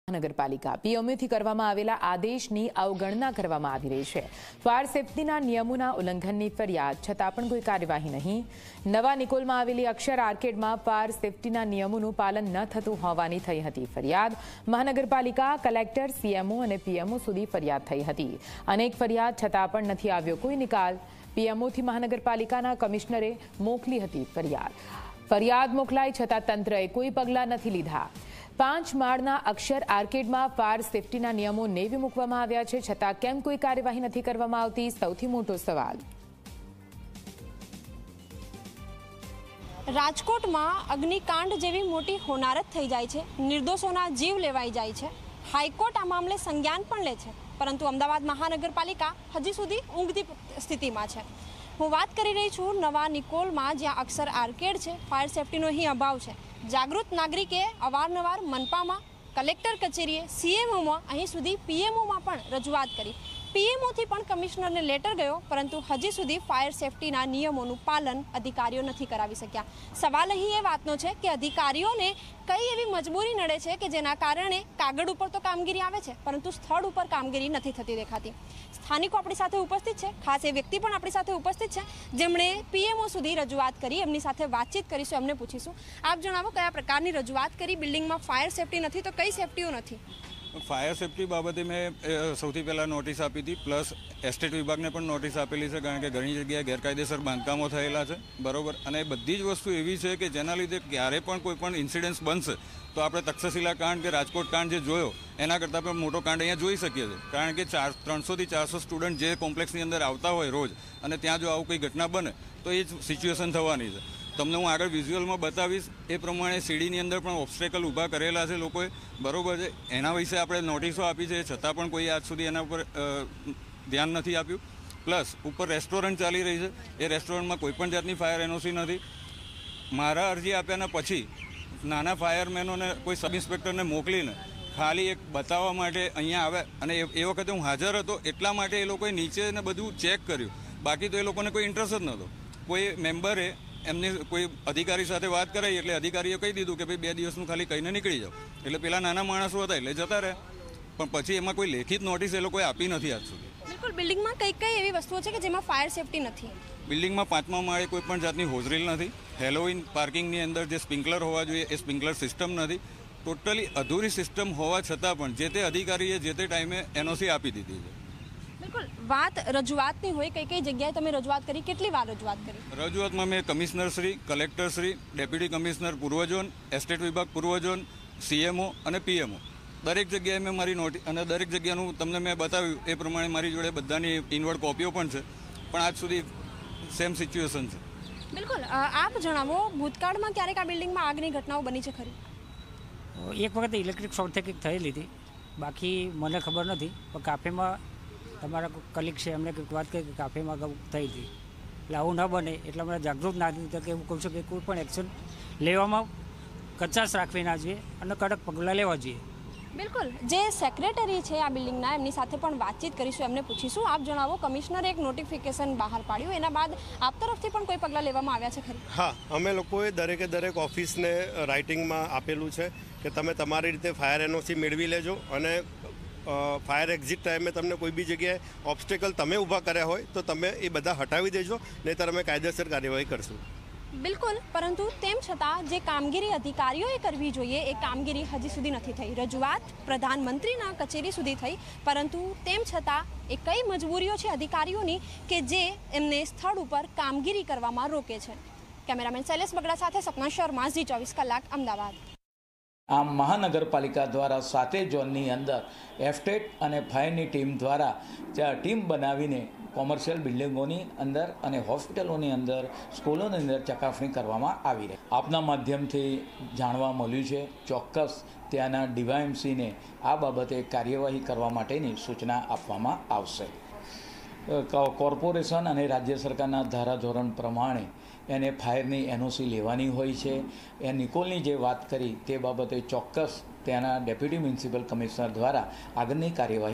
Ahmedabad:નવા નિકોલમાં આવેલી અક્ષર આર્કેડમાં ફાયર સેફ્ટીના નિયમોનું પાલન ન થતું હોવાની થઈ હતી ફરિયાદ
નવા નિકોલમાં આવેલી અક્ષર આર્કેડમાં ફાયર સેફ્ટીના નિયમોનું પાલન ન થતું હોવાની થઈ હતી ફરિયાદ, છત્તાં કોઈ કાર્યવાહી નહીં